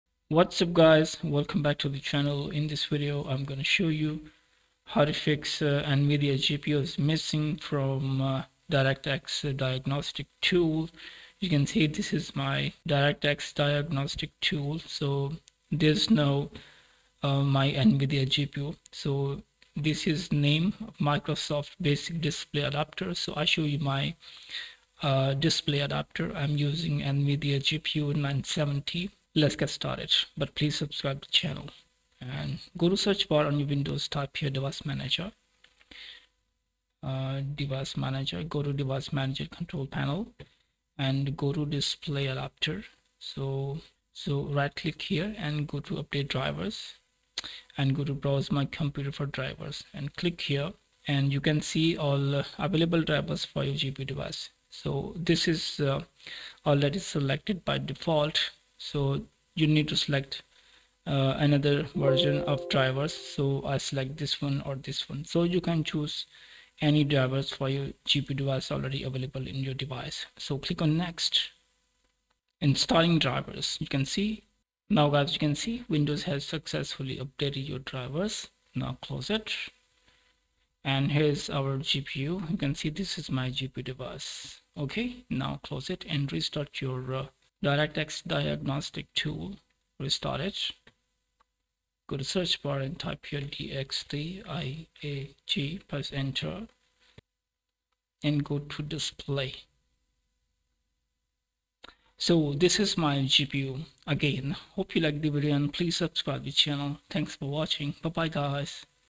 GPU Nvidia is Missing in Directx diagnostic Tool
Hello,
In this video i have shown how to fix GPU Nvidia is Missing in Directx diagnostic Tool.